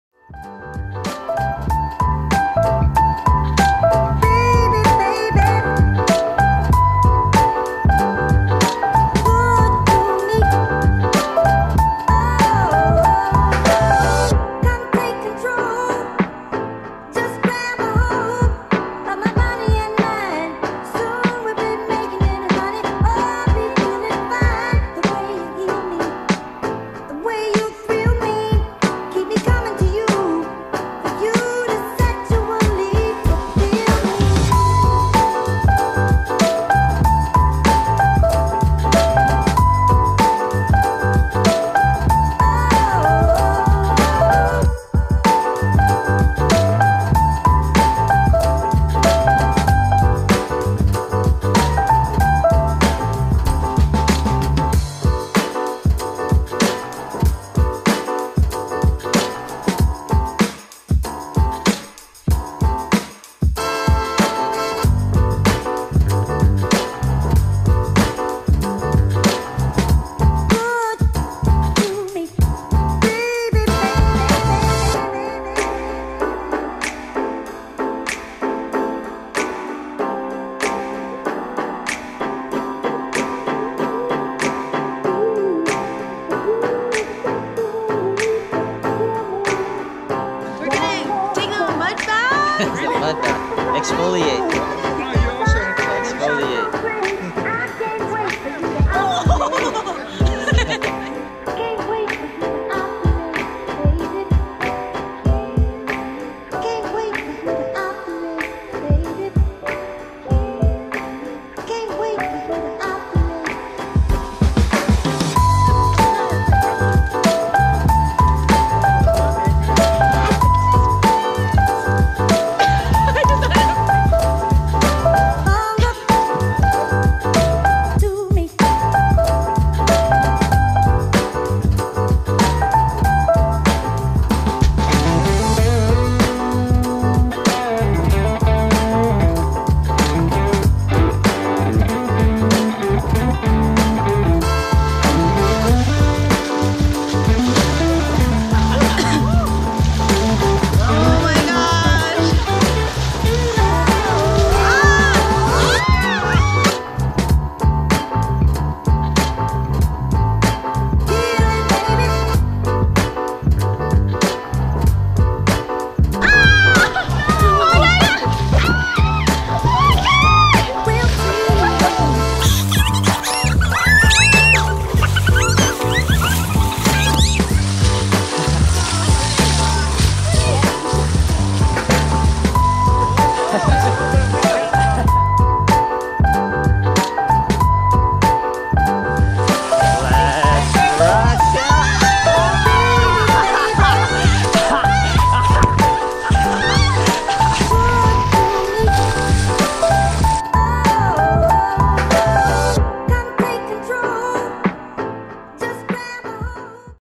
Elephant Freedom Program
Elephant Freedom Program is part of Elephant Nature Park. My group was taken to the mountains of the Karen Village which was about an hour and half away from Chiang Mai.

Spending the day giving back to these gentle giants is an unforgettable experience!  Our group had a family of three elephants, a mom and her two sons age 15 and 21.

We hiked with them, fed them, bathed them in mud, bathed them in the river, then had a water-fight. I felt like big kid spending the day with these beautiful animals and making memories with new friends as well. Hope you enjoy the short video!

* There are multiple Elephant Freedom Programs ( they did not clarify this on their website during booking), so if you plan to go in a group make sure to book it at the same time or they may send your friends off to another village with different elephants.